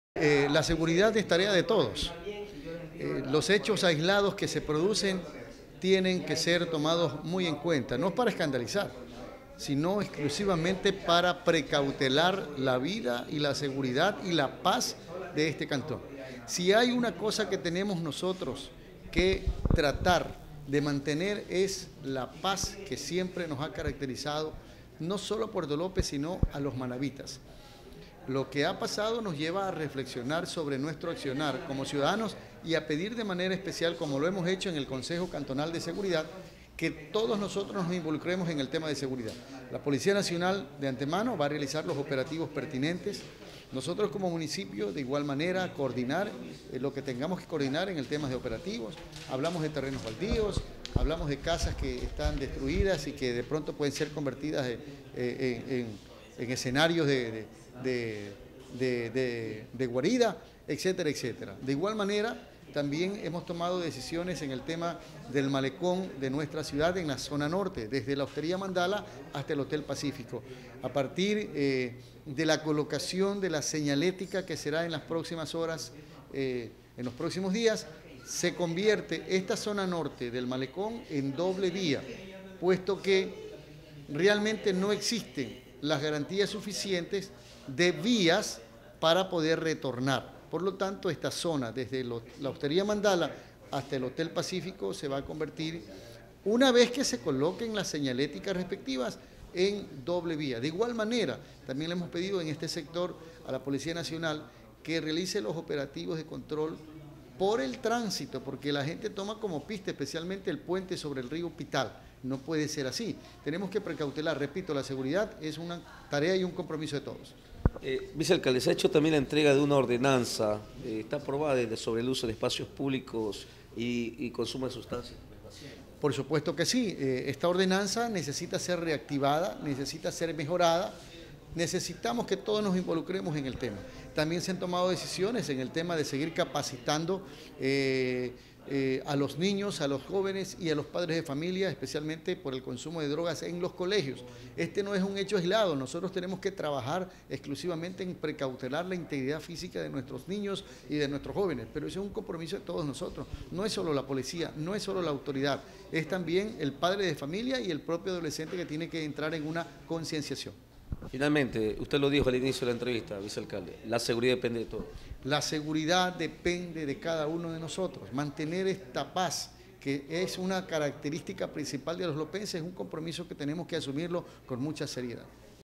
MAURI RICAURTE - SECTOR TURÍSTICO
MESA DE SEGURIDAD SE REÚNE PARA BUSCAR ESTRATEGIA CONTROL Y SEGURIDAD